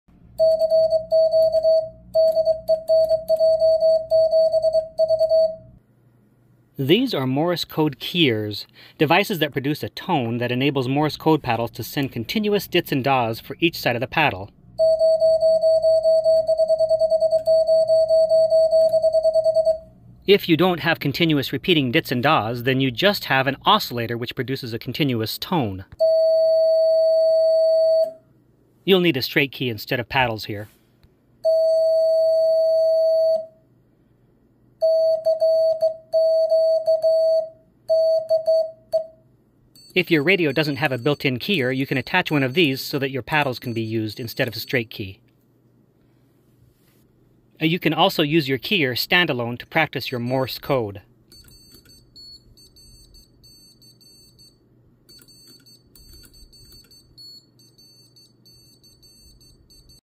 What is a Morse Code Keyer
Morse code keyers have multiple uses.  Some are built into modern ham radios, but some are external stand-alone.